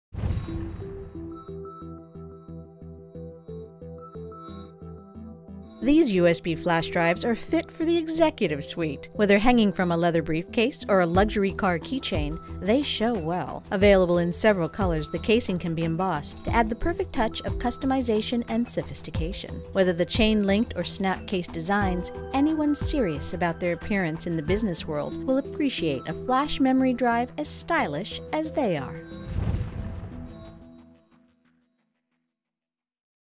Idol Memory Leather Key Chain USB Flash Drive - KSL390401
Sporty, sophisticated and fun all in one
Stylish USB flash drive comes in an attractive, embroider-ready snap case. Polished chrome accents and clip attachment create a sophisticated, practical accessory. Check out key chains USB flash drive from Idol Memory.